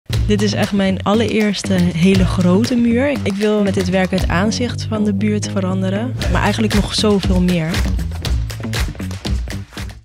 Nadia, onze support heb je | Nationale-Nederlanden is supporter van talent - teaser
Kom maar op met die muur!
Je werk levensgroot op een muur middenin de stad... Veel beginnende street artists kunnen hier alleen maar van dromen. Maar voor aanstormend talent Nadia van Luijk werd het werkelijkheid. Zij won de ontwerpwedstrijd die Nationale-Nederlanden in samenwerking met het Mauritshuis organiseerde. Op een muur van 12 meter hoog, midden in de stad, bracht ze haar ontwerp tot leven. Ze liet zich inspireren door een werk van Jan Davidsz de Heem uit het Mauritshuis: Vaas met bloemen. Met support van Nationale-Nederlanden veranderde ze zo het straatbeeld van het Transvaalkwartier in Den Haag.

De muurschildering van Nadia is onderdeel van een serie van vijf Mauritshuis Murals die gemaakt werden in het kader van de 200e verjaardag van het museum. Nationale-Nederlanden is partner van het museum en helpt op deze manier om zoveel mogelijk mensen de kunst uit het Mauritshuis te laten ontdekken. Wil je meer weten over Mauritshuis Murals? Kijk dan op